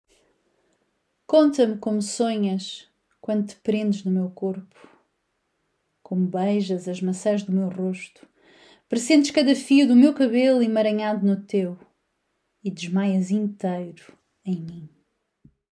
Conta-me como sonhas
Poema 95 - Eros Inversos Psiquê
Josepha Pais
Voz de MJC